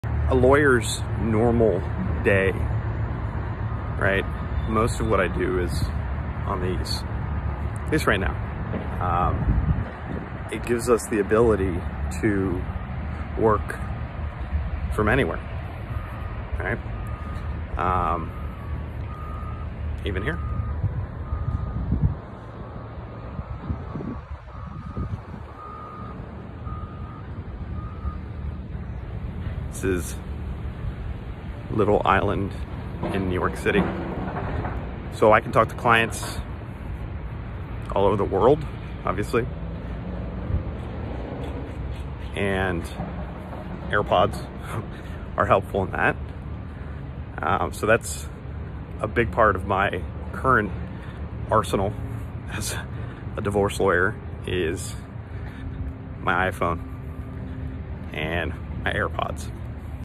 A day in the life of a lawyer
A day in the life of a divorce lawyer. William Foley is a Florida divorce lawyer with a remote divorce law firm. As a divorce lawyer in 2021, opportunities for remote work are more than they have ever been. Working from home, coffee shops, the "actual office", courthouses, airports, cars, or New York City - it's all fair game now!

Please feel free to like this video and comment, as we are always looking for suggestions for more helpful content!